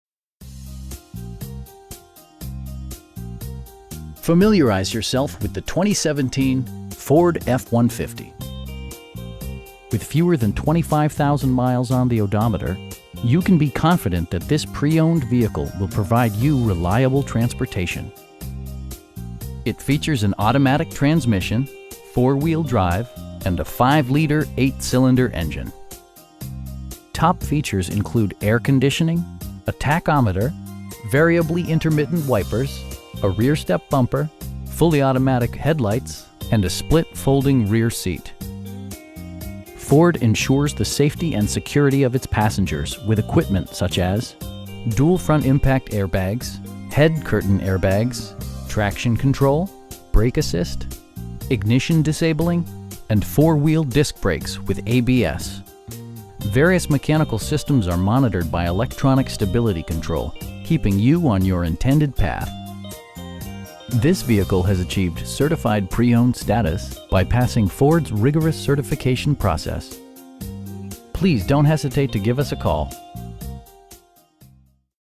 2017 Ford F-150 XLT in Lancaster, CA 93534
Familiarize yourself with the 2017 Ford F-150. With fewer than 25,000 miles on the odometer, you can be confident that this pre-owned vehicle will provide you reliable transportation. It features an automatic transmission, 4-wheel drive, and 5 liter 8 cylinder engine. Top features include air conditioning, a tachometer, variably intermittent wipers, a rear step bumper, fully automatic headlights, tilt steering wheel, and a split folding rear seat. Ford ensures the safety and security of its passengers with equipment such as: dual front impact airbags, head curtain airbags, traction control, brake assist, ignition disabling, and 4 wheel disc brakes with ABS. Various mechanical systems are monitored by electronic stability control, keeping you on your intended path. This vehicle has achieved Certified Pre-Owned status, by passing Ford's rigorous certification process. You will have a pleasant shopping experience that is fun, informative, and never high pressured. Please don't hesitate to give us a call.